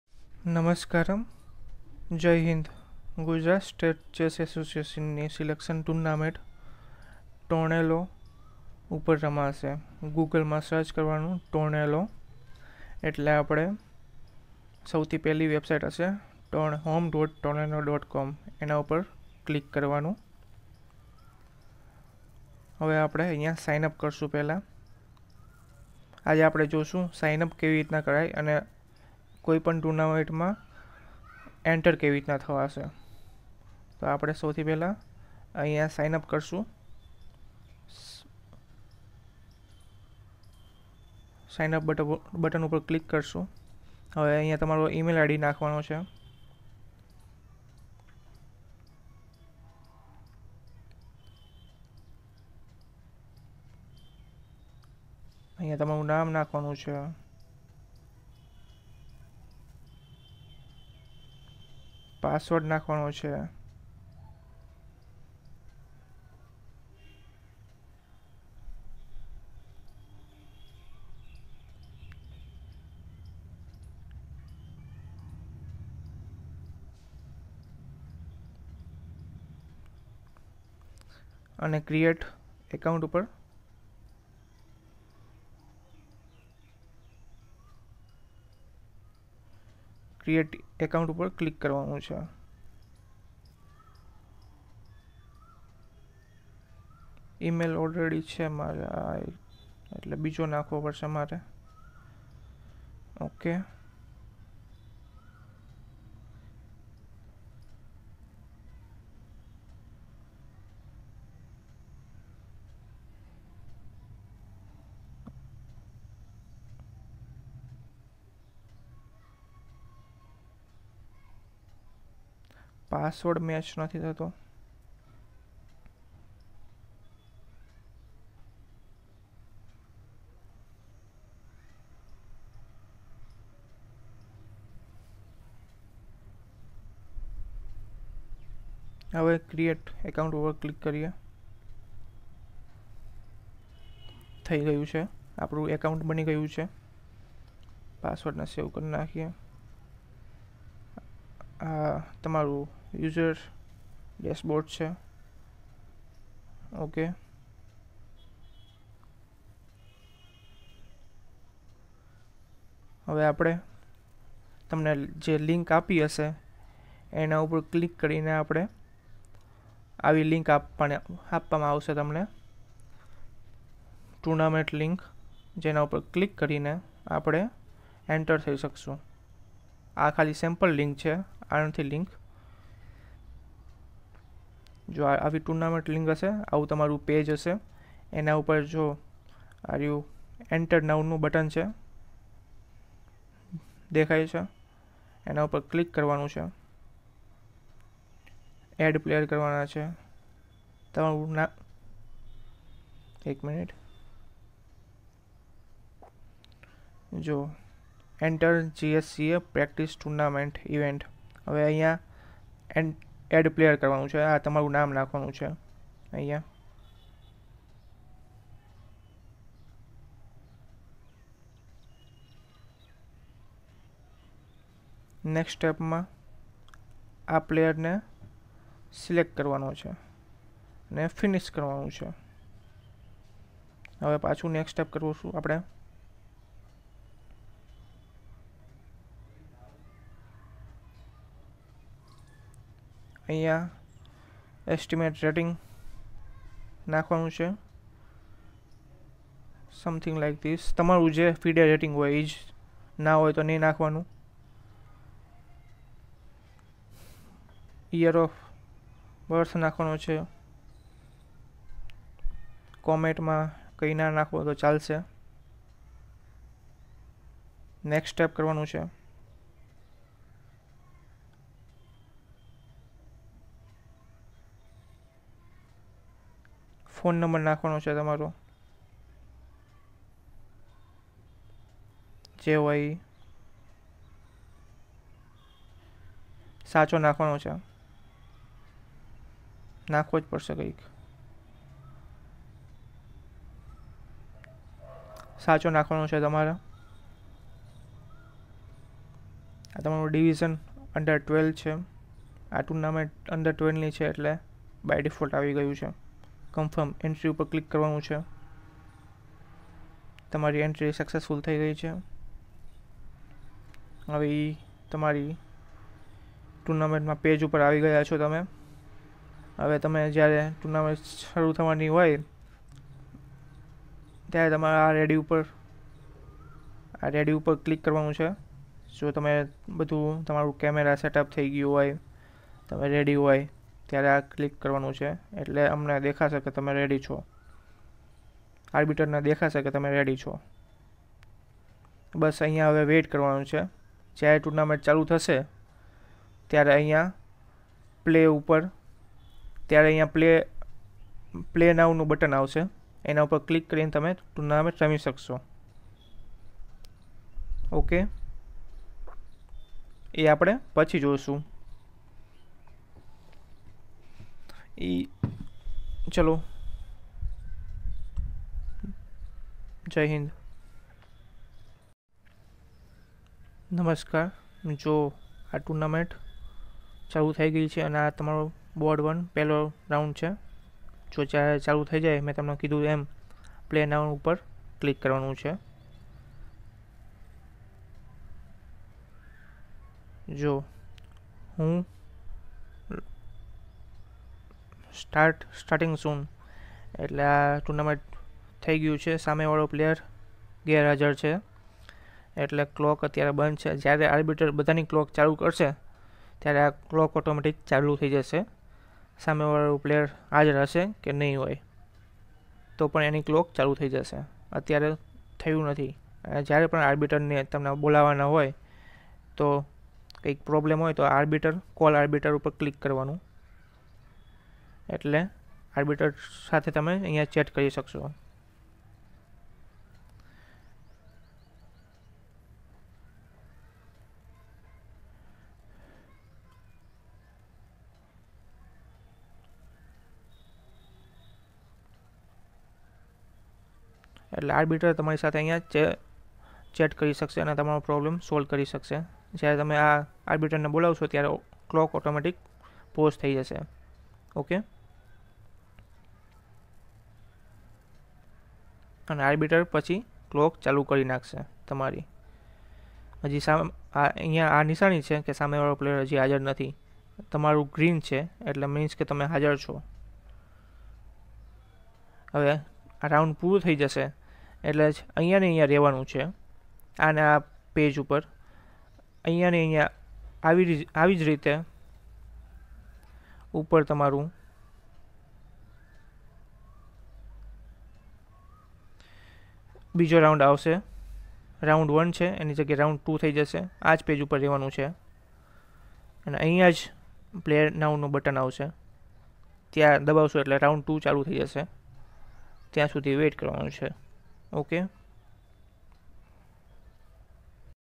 Use LAPTOP Only | How to Sign-up in Tornelo Website and How to Enter in Ches Tournament and Play
For GSCA Chess Tournament, Use LAPTOP only. No Mobile